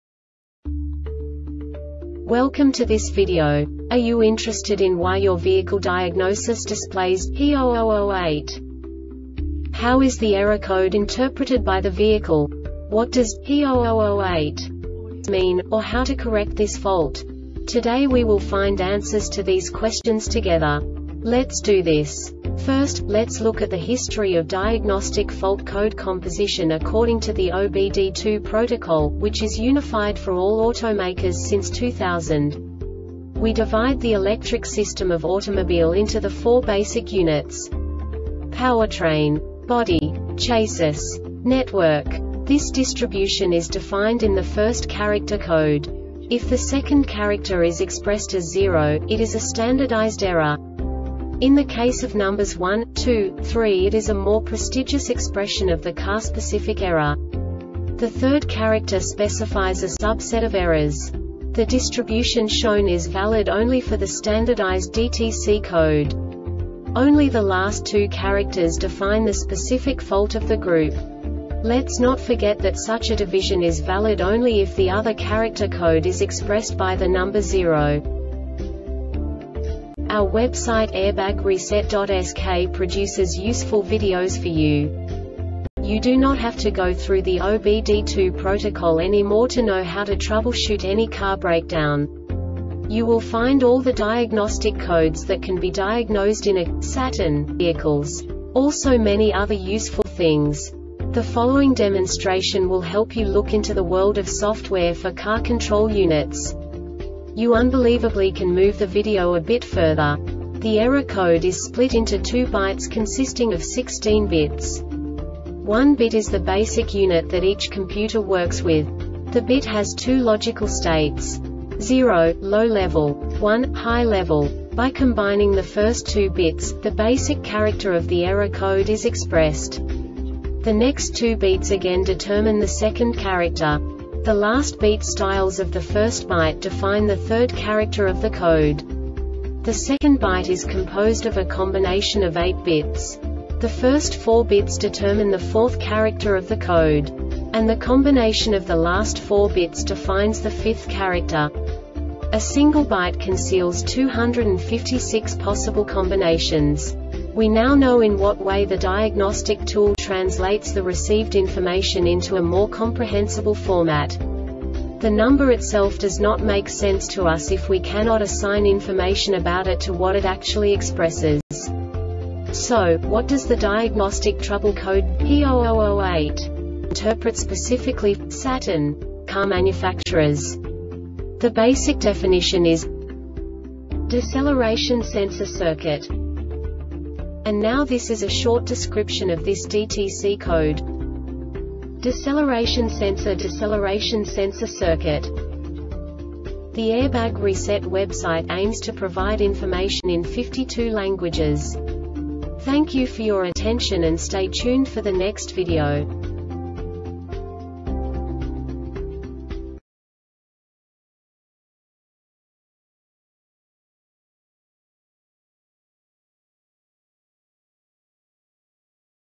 DTC Saturn P0008 Short Explanation
Contents:
0:21 Basic DTC analysis according to OBD2 protocol standard.
1:48 Insight into programming
2:58 A diagnostically understandable description of the Diagnostic Trouble Code
3:05 Basic error definition

Make:
Saturn
Code:
P0008
Definition:
Deceleration Sensor Circuit
Cause:
Deceleration sensorDeceleration sensor circuit